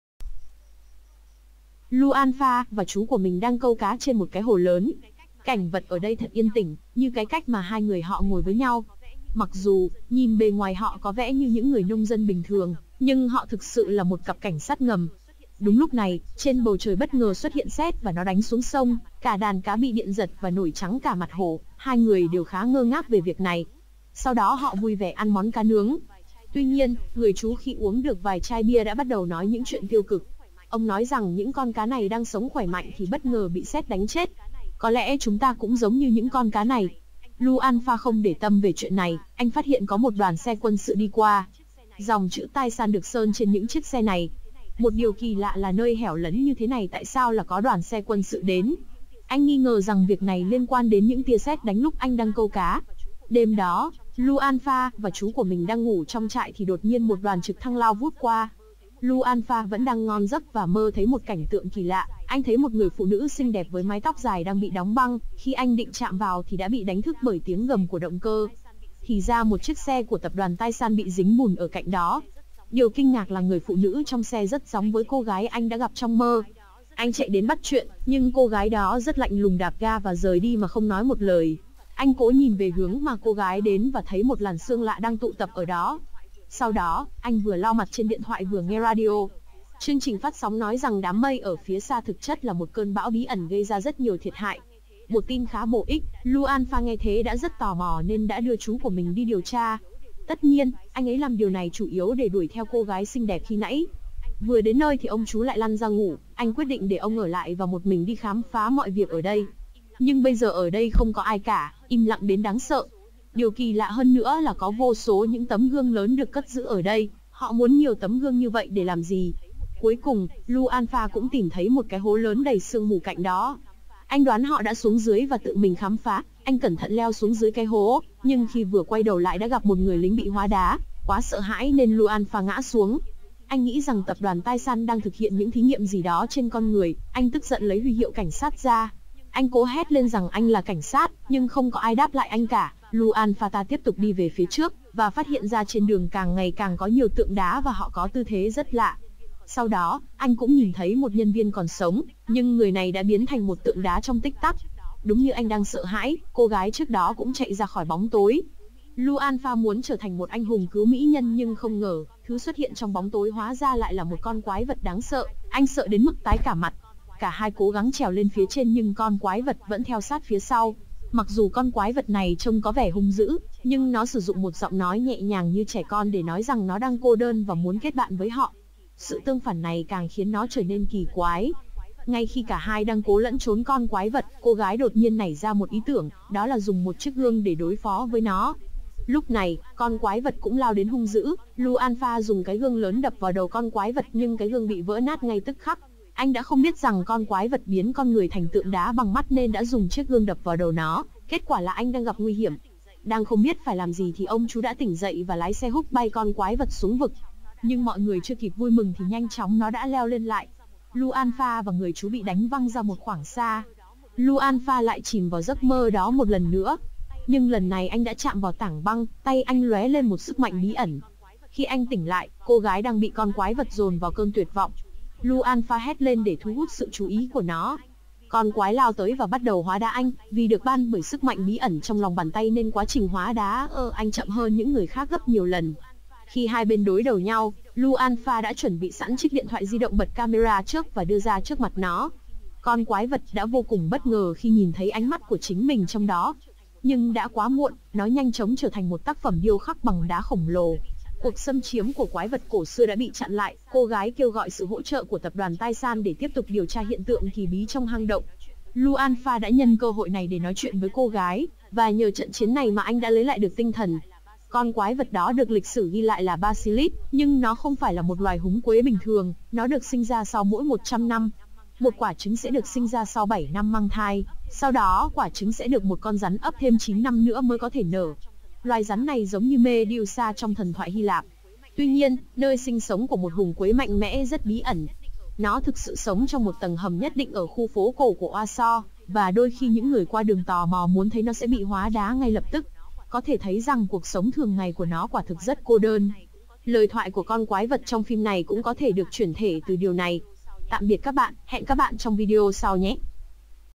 Rút Gọn Phim : Quái Vật Ngàn Năm Bị Đánh Thức Và Hóa Đá Mọi Thứ | Phim Tâm Lý - Hài Hước
Nội Dung Phim :Quái Vật Nghàn Năm Bị Đánh Thức Và Hóa Đá Mọi Thứ
f